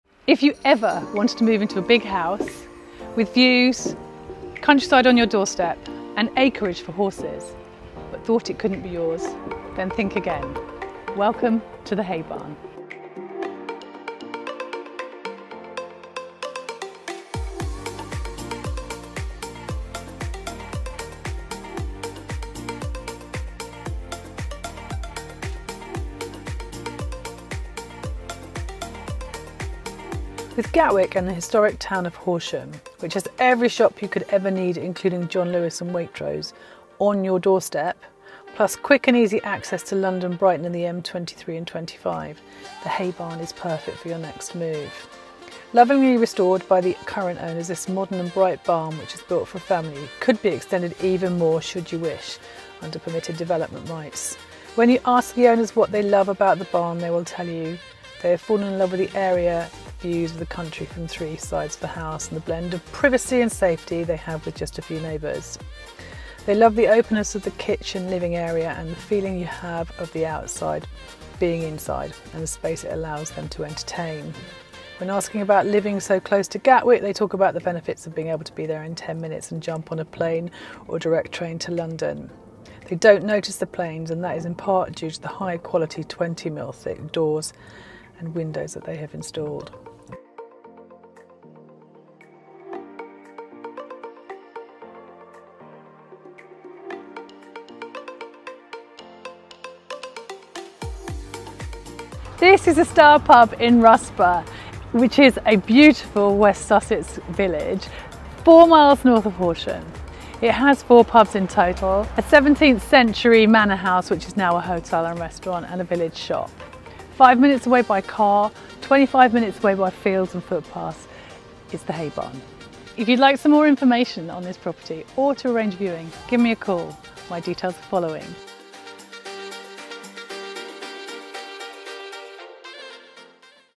Stunning house with views and countryside on your doorstep.
Lovingly restored by the current owners this modern and bright barn which is built for family living.